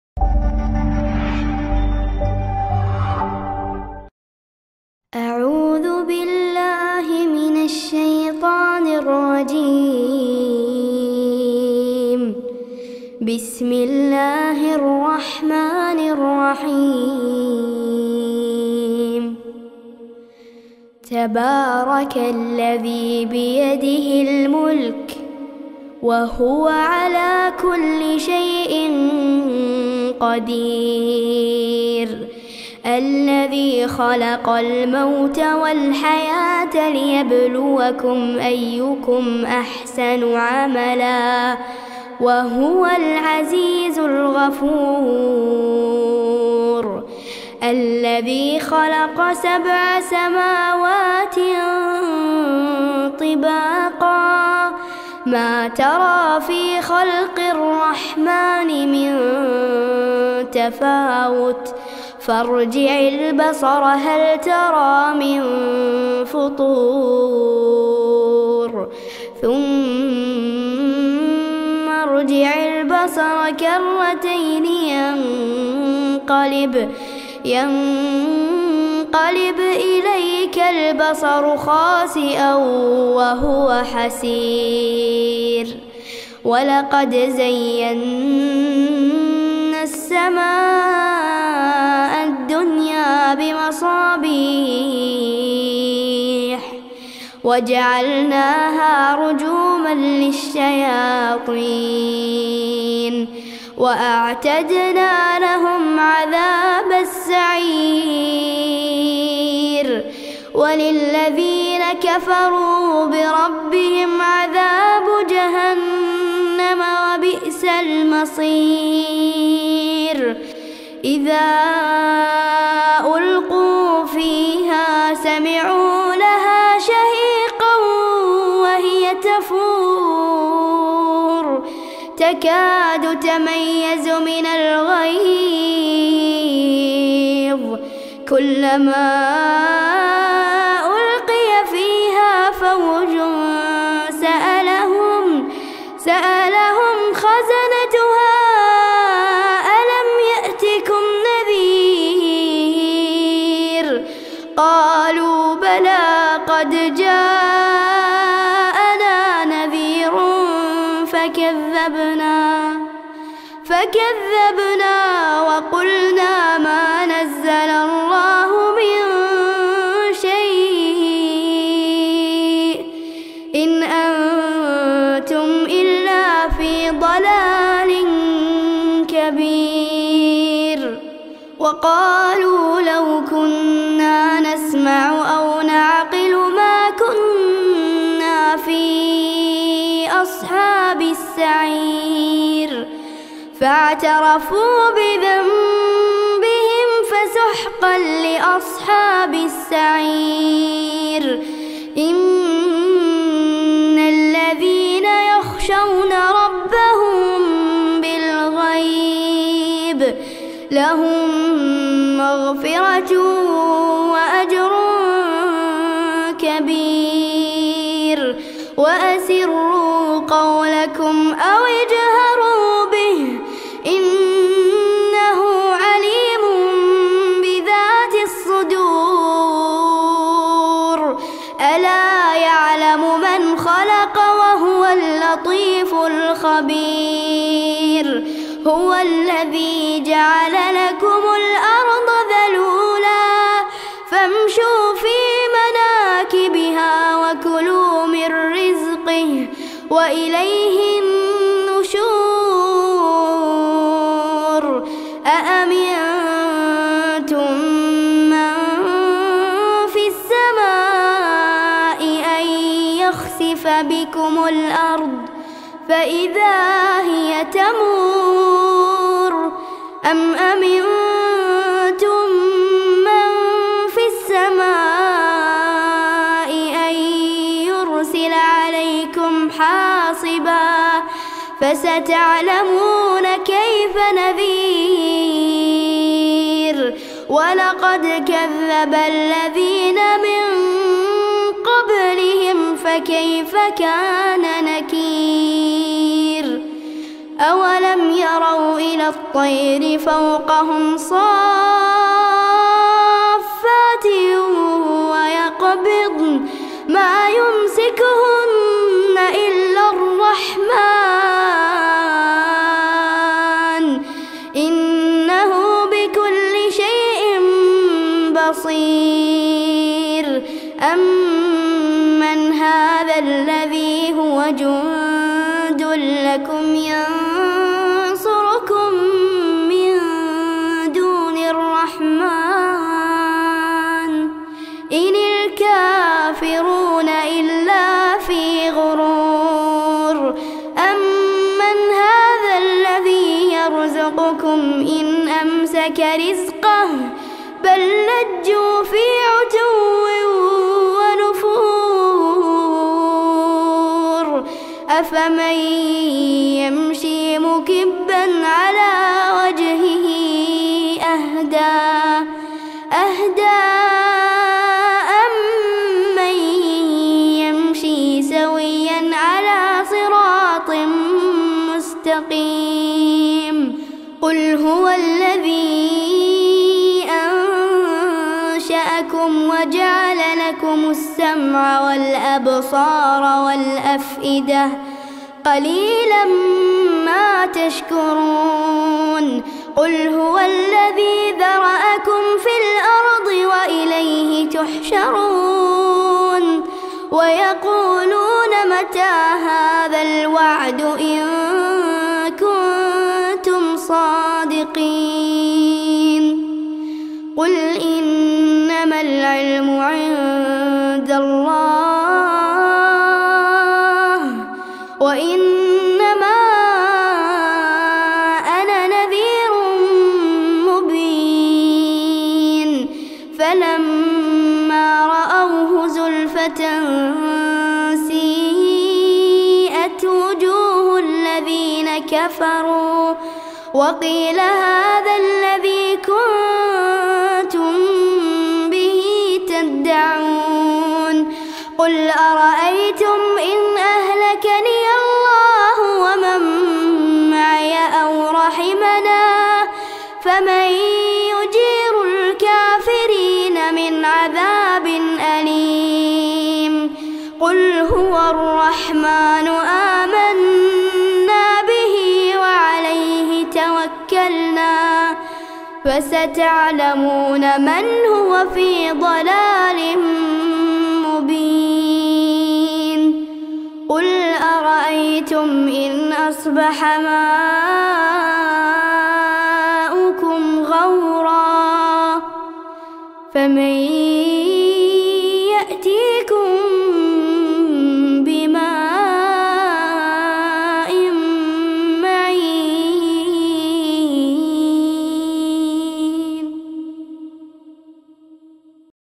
Surah Al-Mulk Utsman Al-Hadad Terjemahan Indonesia dan Inggris
Surat Al Mulk Merdu Menyentuh Hati | Teks Arab Latin dan Terjemahan Indonesia dan Inggris oleh Utsman Al Haddad

Like Fanspage IQRO Pedia